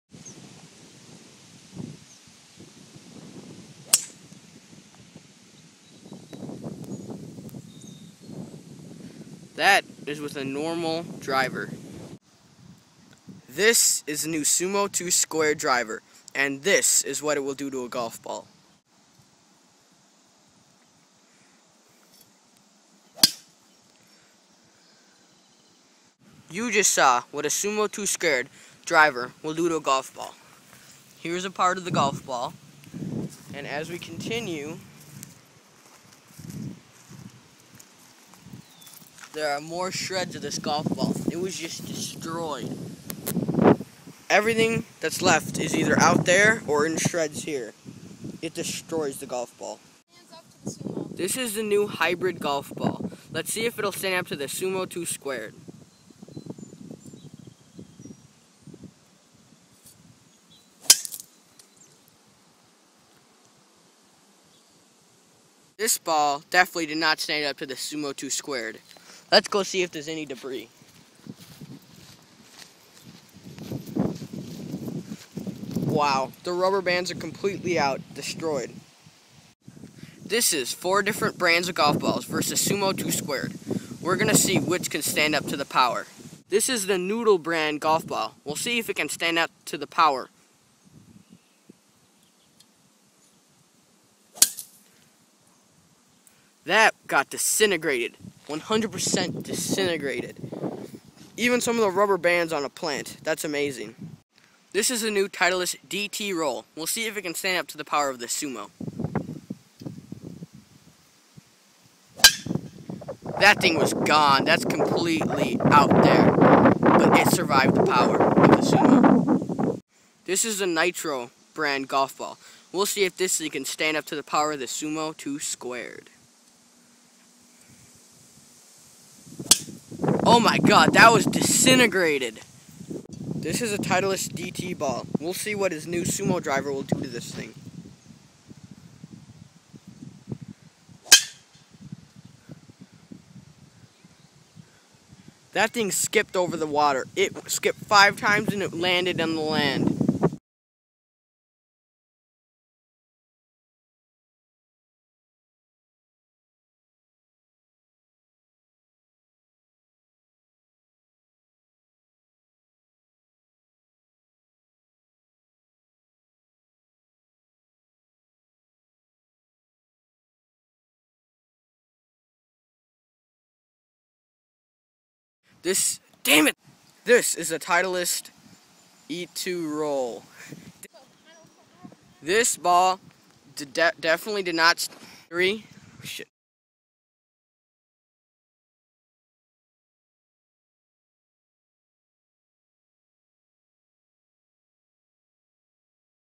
Exploding Golf Balls
These events occurred after danny decided he needed new black golf shorts (modeled in the video). We found these golf balls in a small golf shop for 3 bucks a piece and we couldn't pass the opportunity. Enjoy!